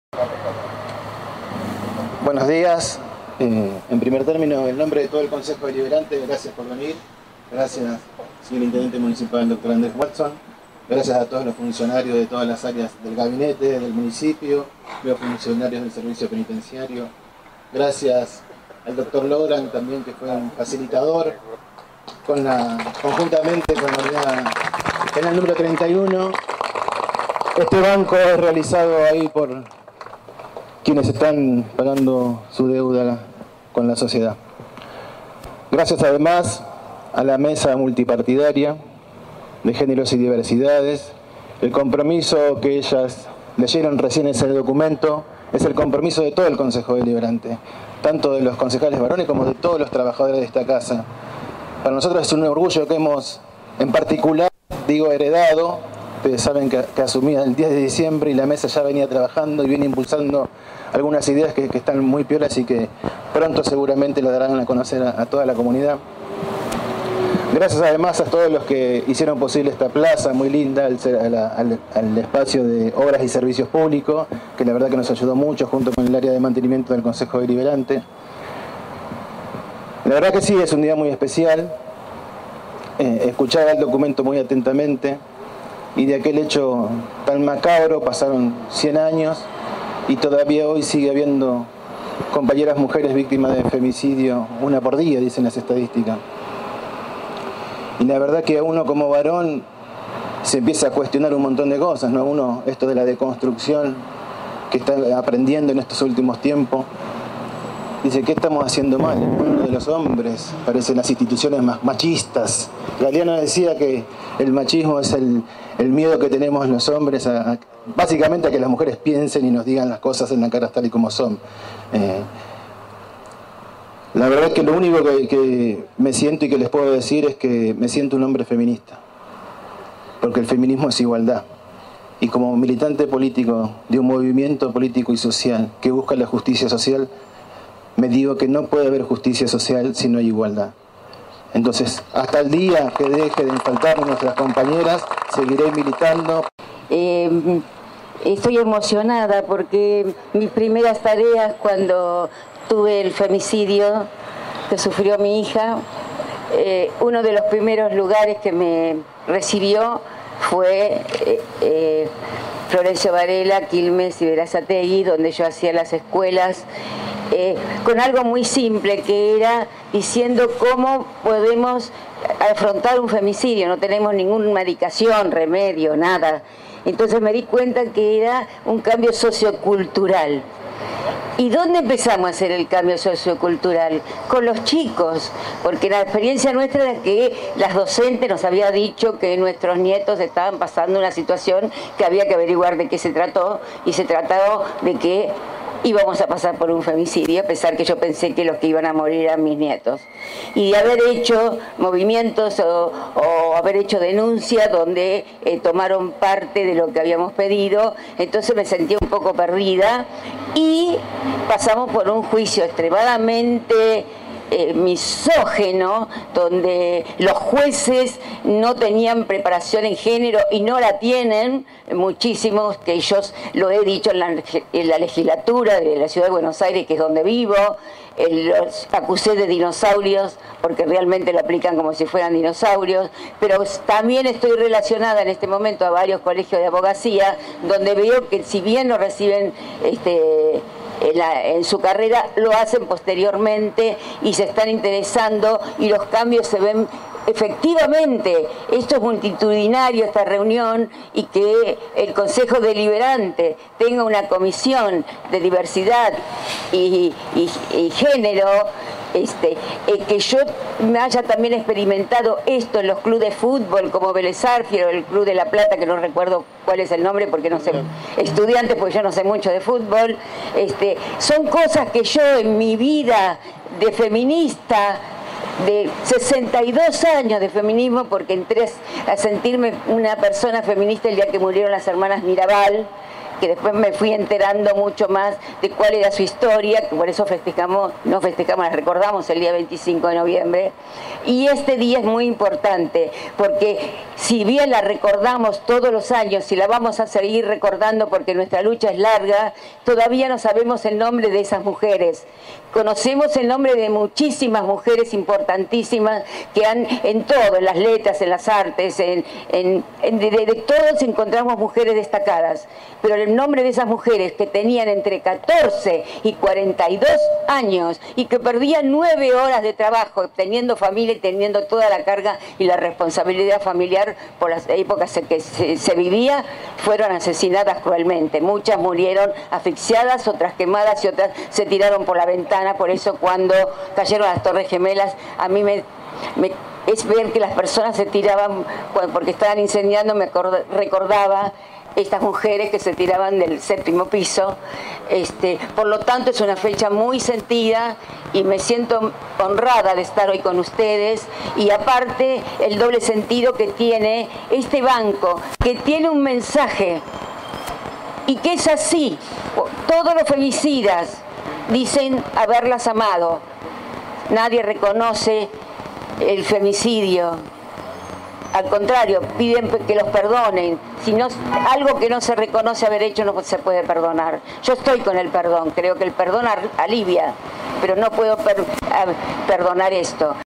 ERDS: "Día internacional de la mujer", acto en el HCD Varelense
Elradardelsur.ar 08/03/2022
El intendente Andrés Watson encabezó el acto, en la mañana de hoy, en el marco de la conmemoración del Día Internacional de la Mujer.
Bajo la coordinación de la Mesa Multipartidaria de Géneros y Diversidad del Honorable Concejo Deliberante de Florencio Varela, espacio que nuclea a las Concejalas de todos los bloques políticos, se desarrolló
En la oportunidad se emplazó en los jardines de la Plaza de la Democracia (Contigua a la sede del departamento legislativo) un banco rojo para visibilizar la violencia que se ejerce contra las mujeres y se desplegó un telón violeta en el lateral del edificio que contiene el recinto deliberativo con la leyenda “Vivas, libres y sin miedos nos queremos! Ni una menos!”. El banco rojo fue confeccionado por internos de la Unidad Penal Nº 31 del Complejo Penitenciario Florencio Varela perteneciente al Servicio Penitenciario Bonaerense